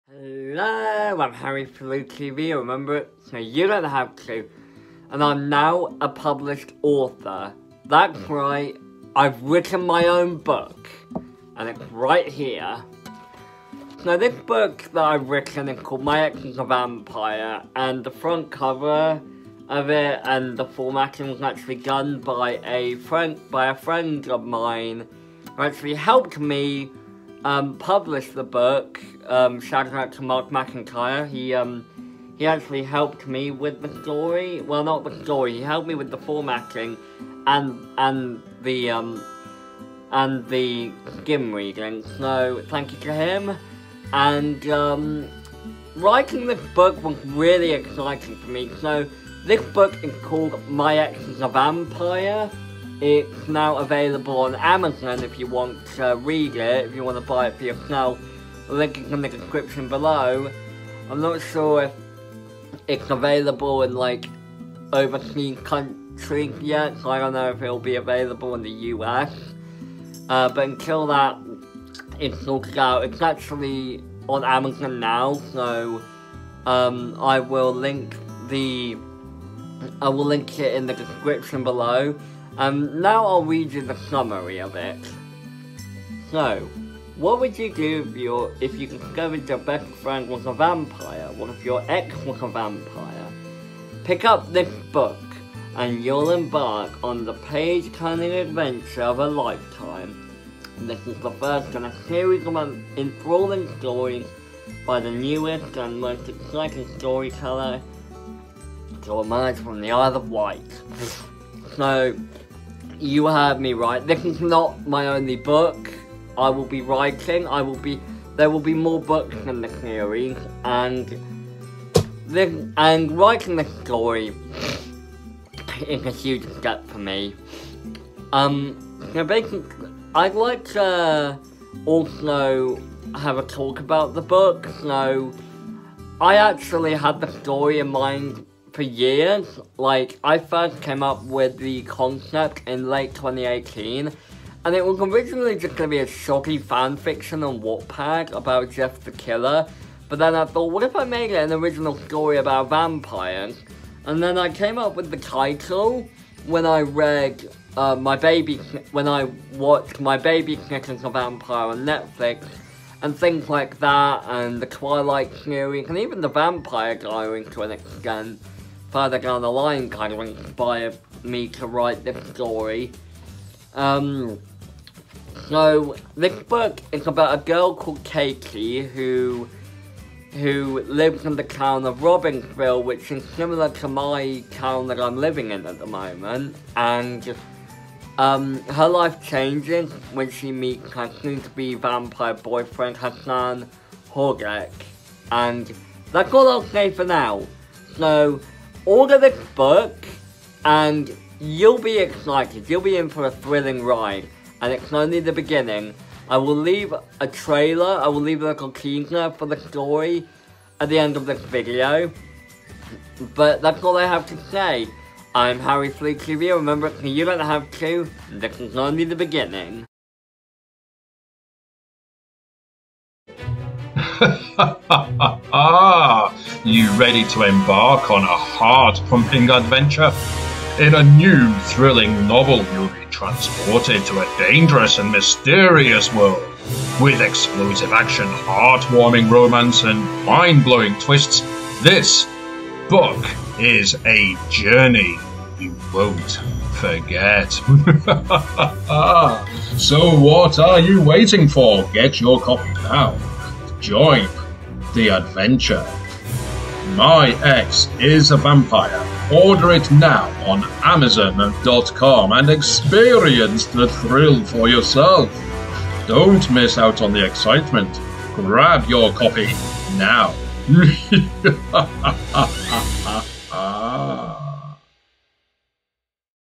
Update￼, 29/03/23: NEW BOOK
What would you do if you discovered your best friend was a vampire? what if your ex is a vampire?
“My ex is a vampire” tells the story of Katey Jarvis, as she meets her vampire boyfriend Hassan Hodek and things seem perfect when they fall in love, but what comes next is a tale of horror and bloodshed,

Pick up this book and you'll embark on the page turning adventure of a lifetime. This is the first in a series of enthralling stories by the newest and most exciting story teller to emerge from the Isle of Wight.